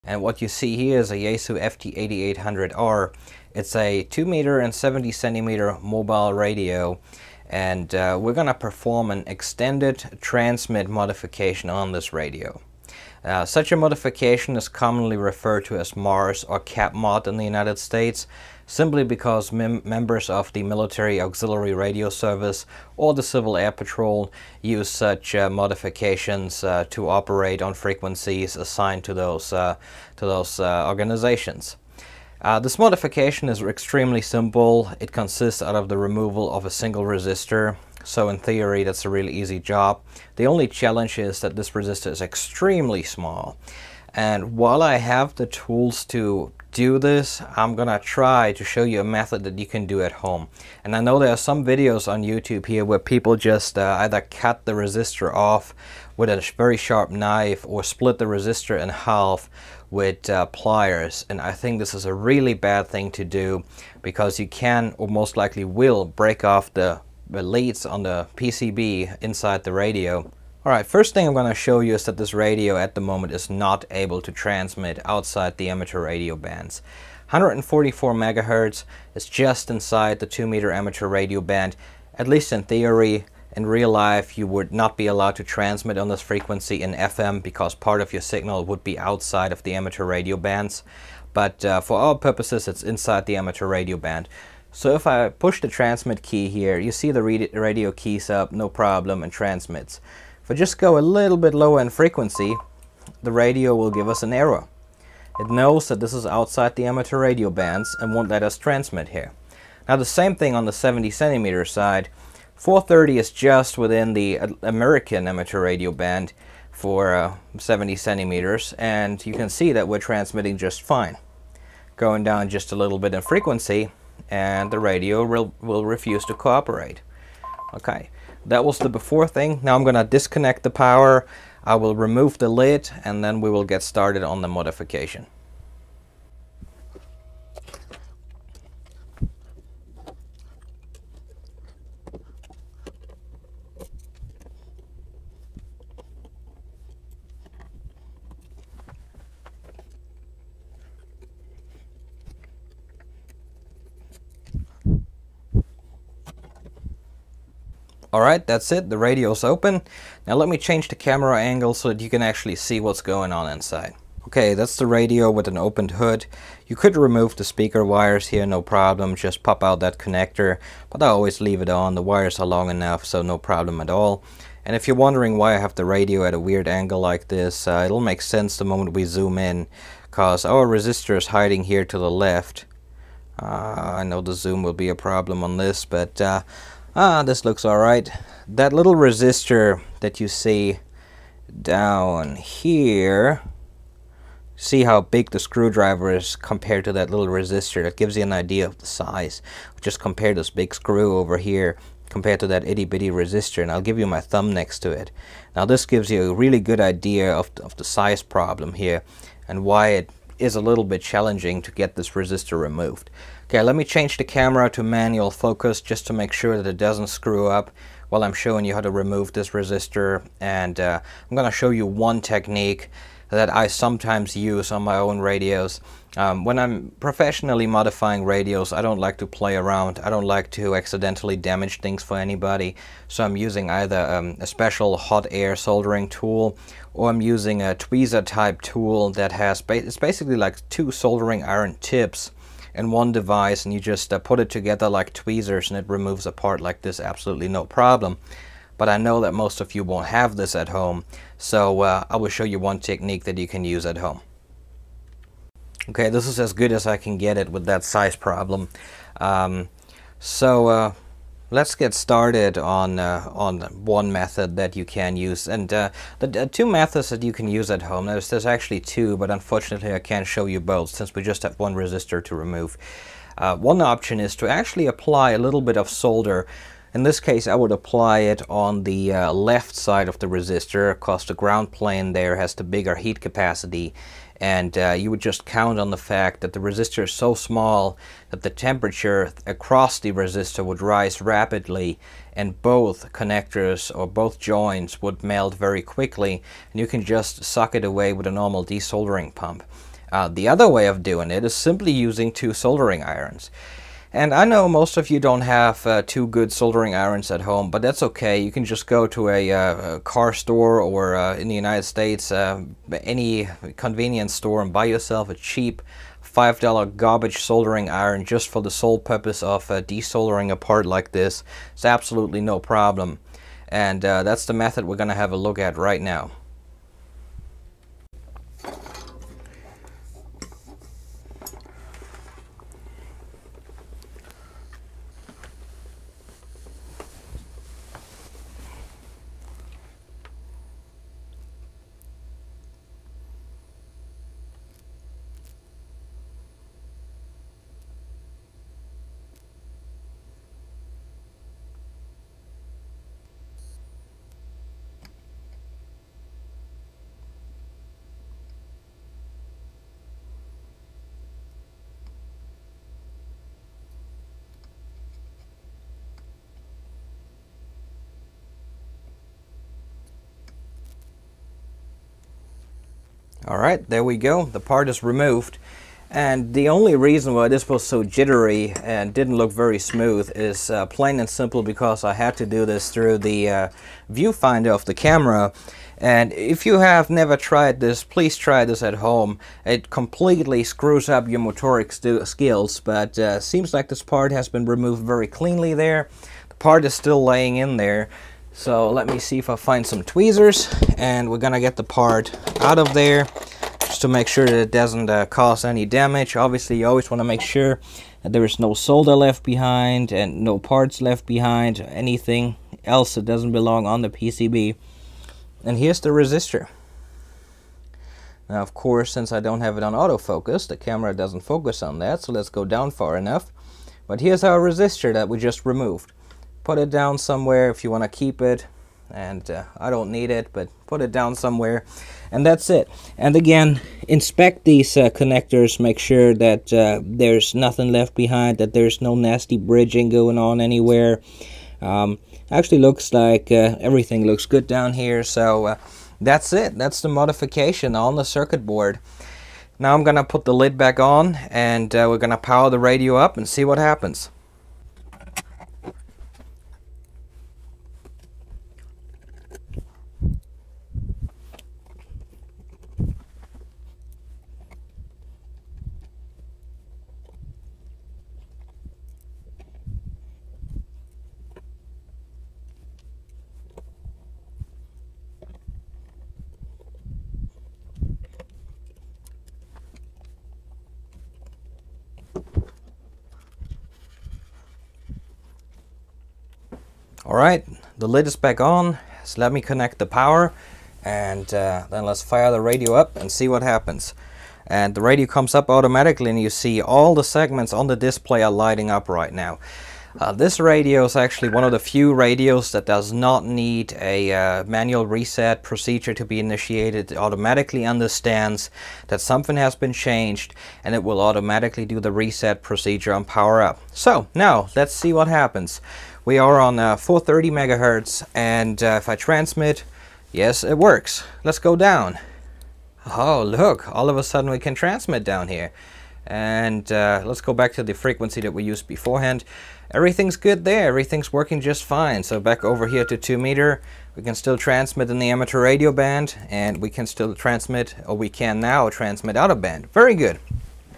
FT-8800 extended TX (MARS/CAP) Mod
In this video, we walk through the simple step-by-step process of performing the MARS/CAP modification on the Yaesu FT-8800 and FT-7900 series radios, including the FT-8800R and FT-7900R. This easy at-home mod allows your radio to transmit outside the standard amateur radio bands-covering 137-174 MHz and 420-470 MHz-making it suitable for driving a transverter or for use in systems like the Military Auxiliary Radio System (MARS) or Civil Air Patrol (CAP). Using an FT-8800R as an example, we show how to open the radio, locate and carefully remove a specific surface-mount resistor, and power-cycle the device to complete the mod. Whether you're expanding your radio's capabilities for hobby or professional use, this guide keeps the process straightforward with clear visuals and tips to avoid common mistakes.

*German Content*
▸My German Channel:  @balticlabor